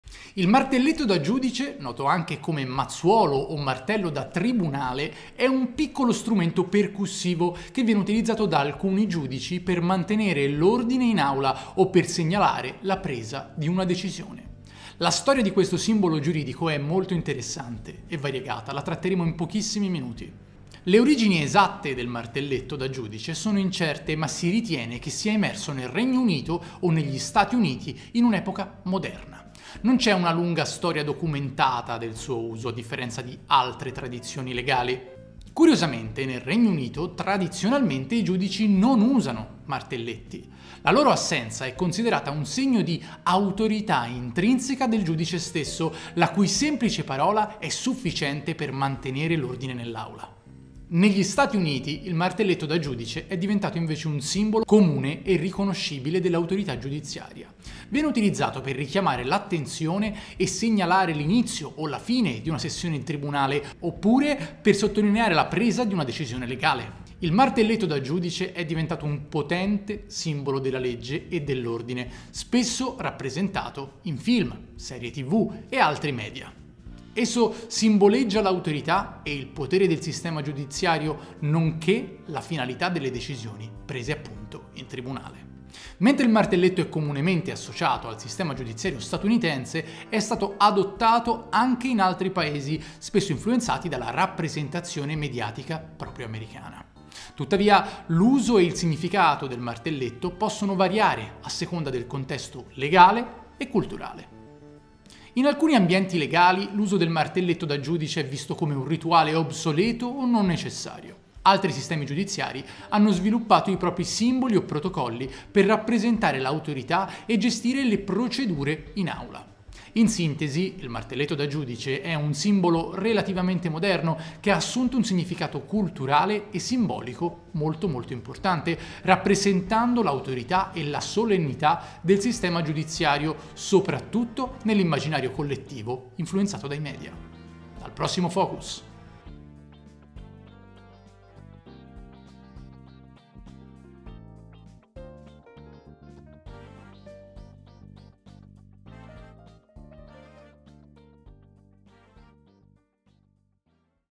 Il martelletto del Giudice
In soli 2 minuti ripercorreremo insieme la storia del martelletto del giudice e verificheremo qual è il suo scopo ed in quali Tribunali si usa tutt'oggi.

Ringraziamenti speciali:

- Maestro M. Compagno: Main theme music